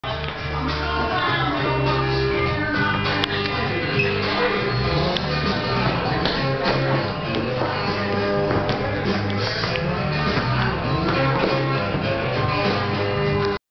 breakin' in vancouver!
After shooting motion capture one day in Vancouver, Canada for Electronic Arts. This is what happens when you drink to much wine and eat too much poutine!  Drunken, sloppy windmills in the Opus hotel lobby!